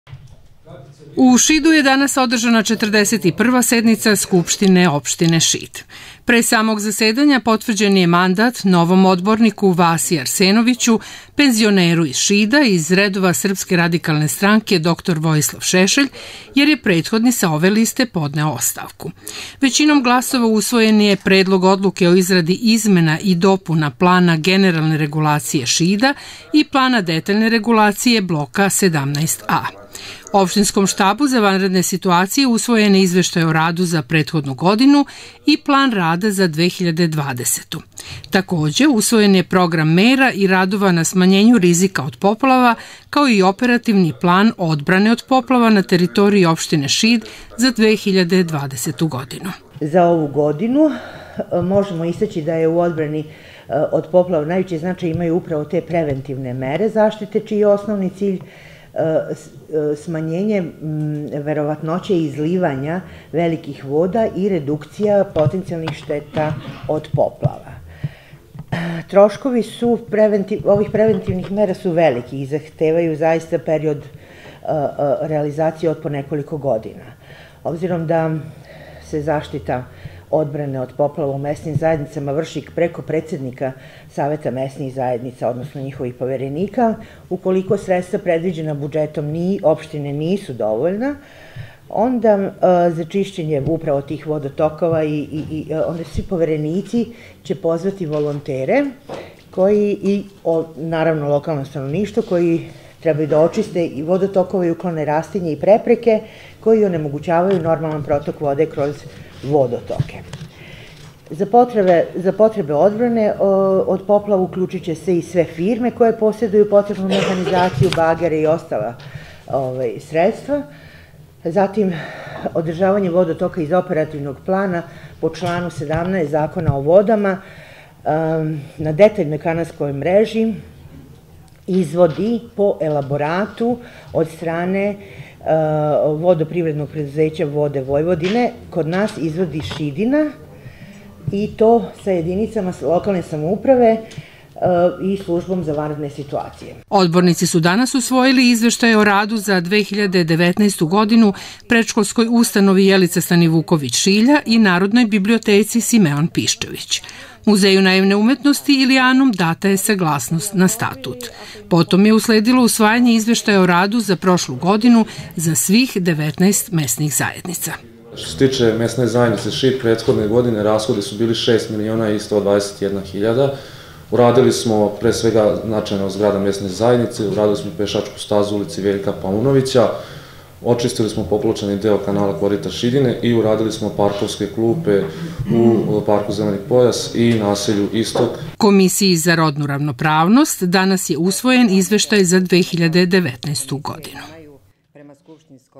Sednica SO Mart
Odbornici Skupštine opštine Šid usvojili su danas na svojoj 41. sednici Odluke o izradi izmena i dopuna Plana generalne regulacije Šida ,  Izveštaj o radu za 2019. godinu i Plan rada za 2020. godinu Opštinskog štaba  za vanredne situacije  opštine Šid kao i   Izveštaje o radu mesnih  zajednica. Usvojeni su i izveštaji o radu u 2019. godini predškolskoj ustanovi "Jelica Stanivuković-Šilja" i Narodnoj biblioteci "Simeon Piščević".